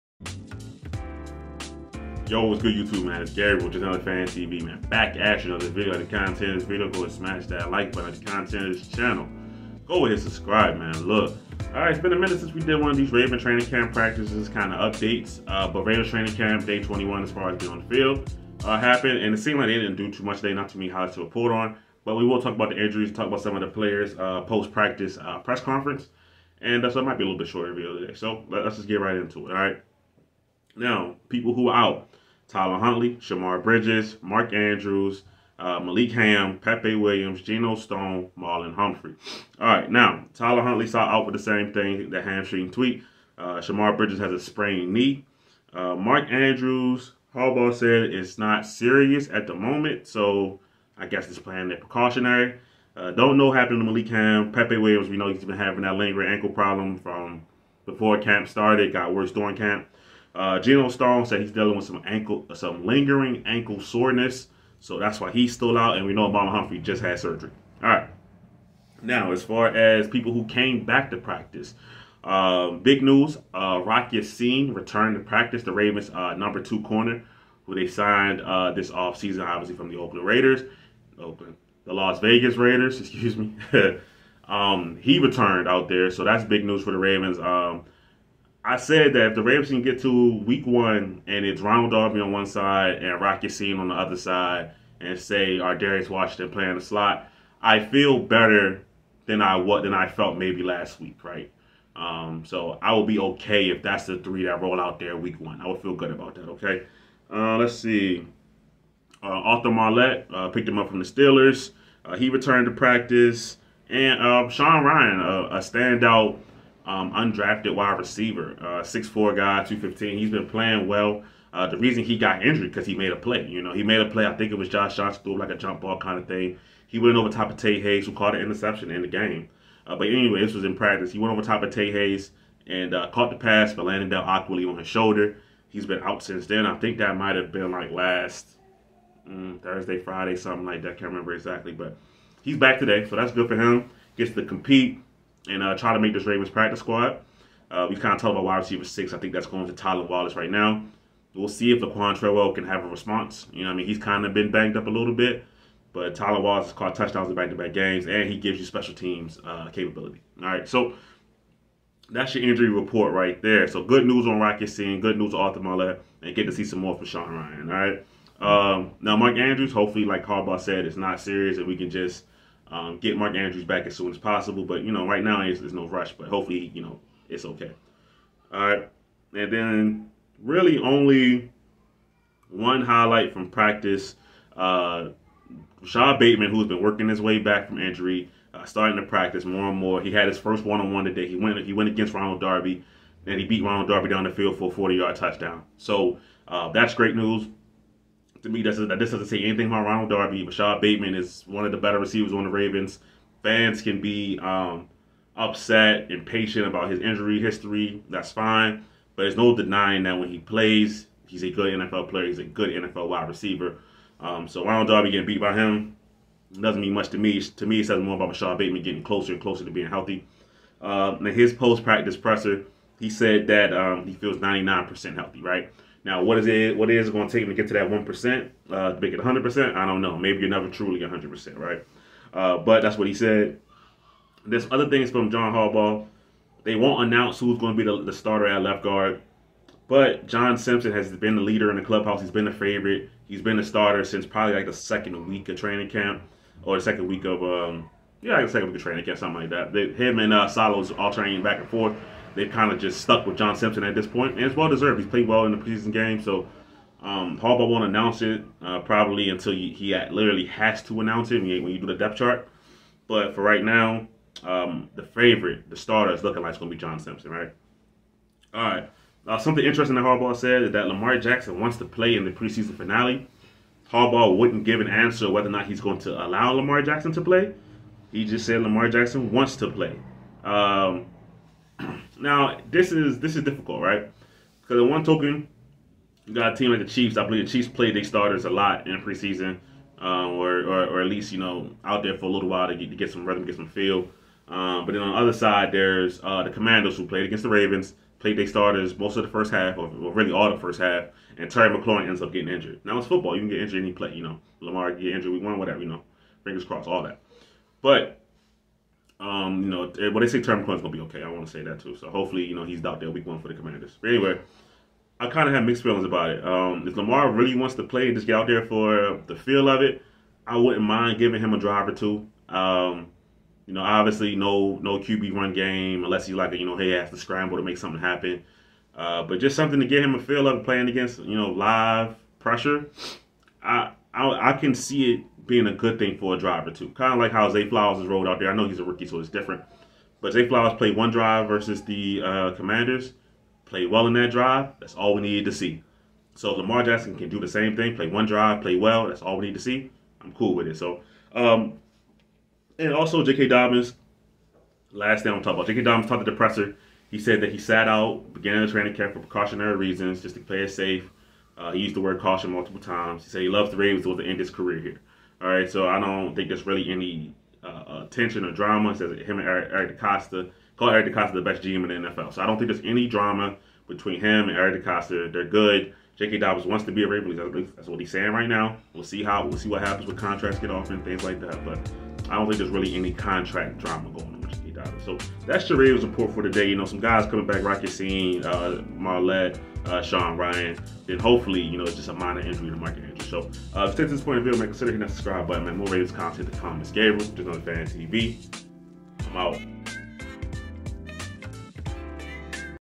Training Camp Day 21: CB Rock Ya-Sin Returns To Practice & Rashod Bateman Gives GREAT Health Update!
The 21st day of Ravens training camp is in the books. Ravens get a key starter back and Rashod Bateman is making plays and feeling better day by day. Let's talk about it!